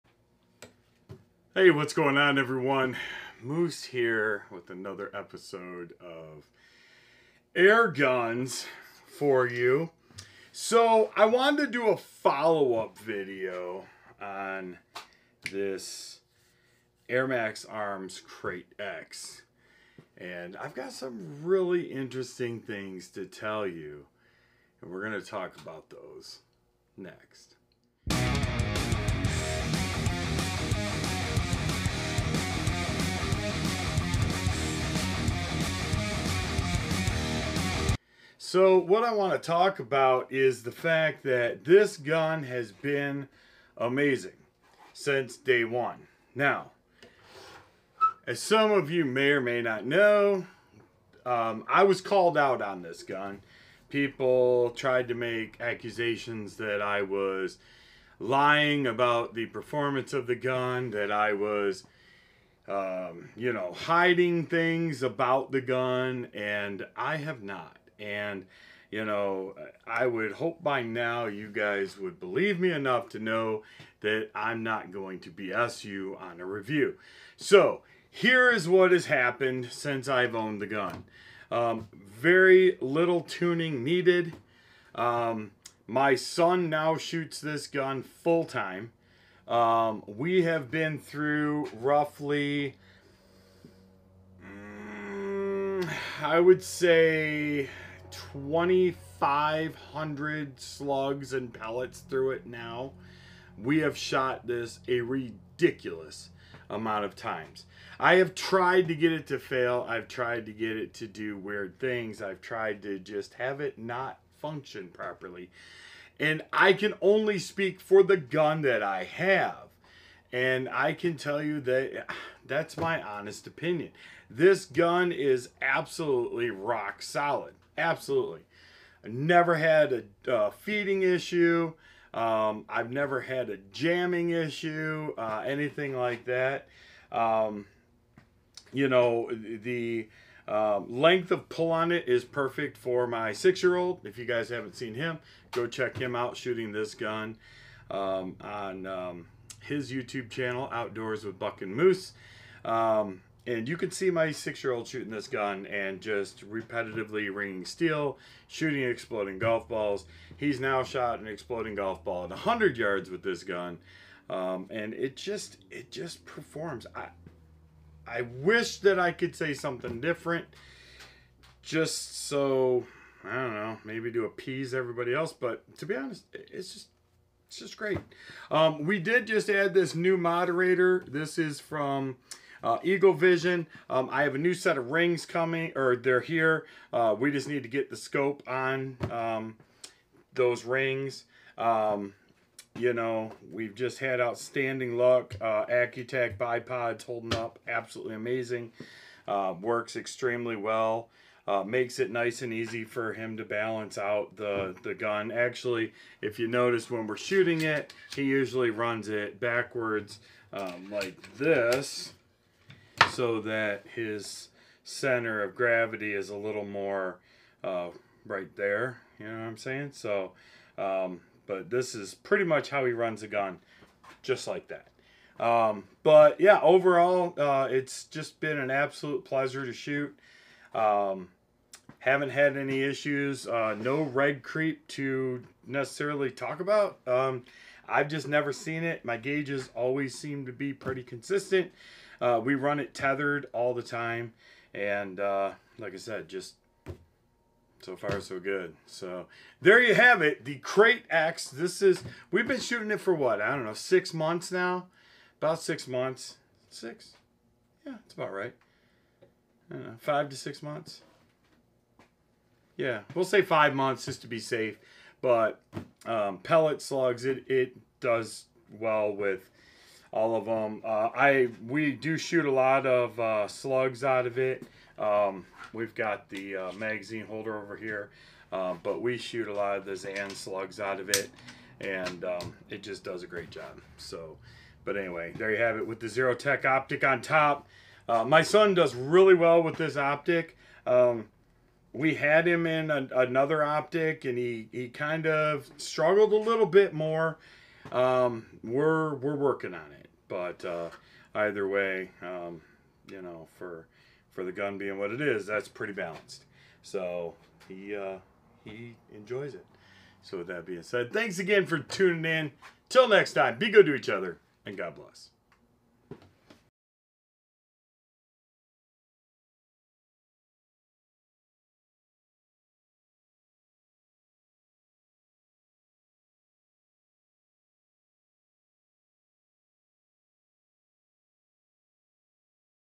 Did the Krait X hold up to 2500+ shots?
Monthly drawings for amazing products like Airguns, scopes, and more

As always please be safe and God Bless